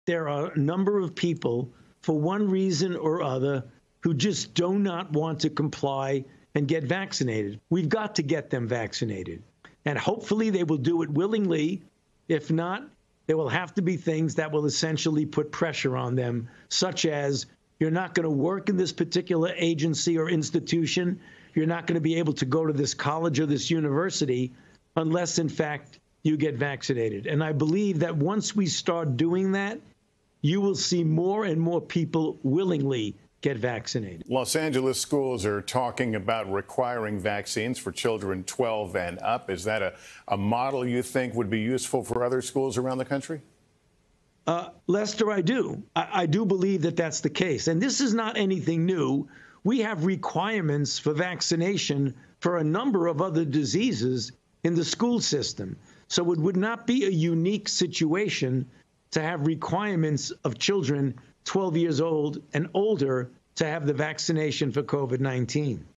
Fauci Talks Putting Pressure On Unvaccinated
We have go to get them vaccinated, you aren't going to work, you can't go to this college